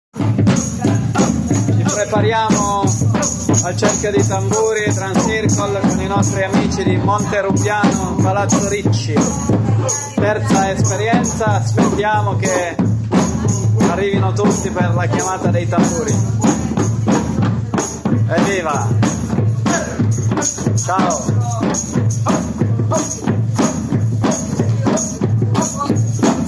Drum Circle a Monterubbiano FM chiamata dei tamburi. Emilio Volgarino cell. 393.0821542
Siamo caldi e pronti per un nuovo cerchio di tamburi e percussioni. si percepisce l'aria frizzante prima di immergerci in un viaggio intenso di ritmi e sensazioni irripetibili. EVviva!!